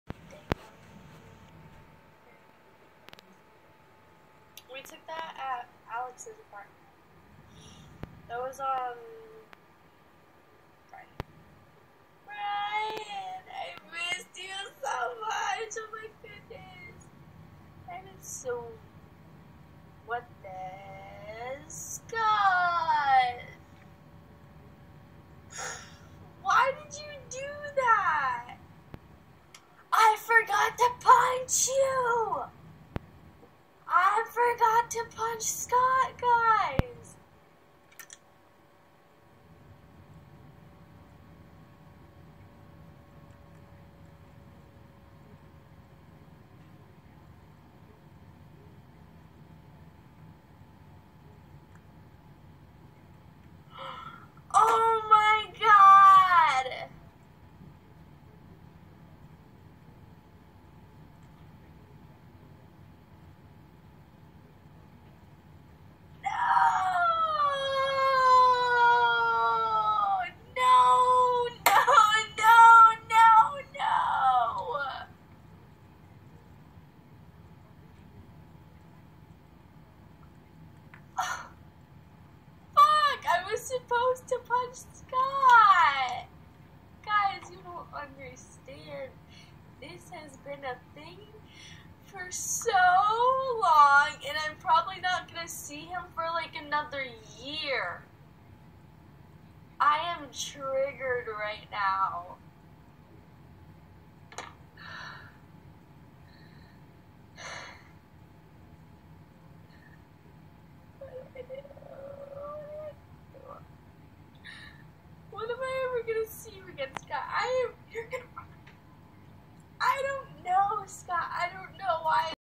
Hannah Stone forgot to punch me
This was just after hannah & i got back from playlist Orlando may 2017. She  used to get so mad at me for tipping her. She didn't like her friends spending money on her. Well I tipped her confetti on younow this day & then she remembered she forgot the punch! The video speaks for itself! It was going to be a nice punch btw lol.